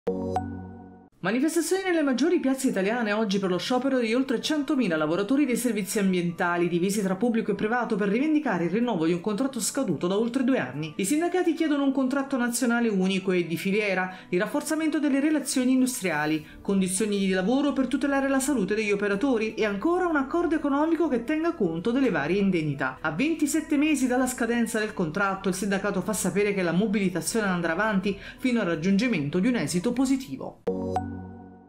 Manifestazioni in piazza: sciopero nazionale e servizi ambientali
Manifestazioni nelle maggiori piazze italiane oggi per lo sciopero nazionale per il rinnovo del contratto di oltre 100mila lavoratori.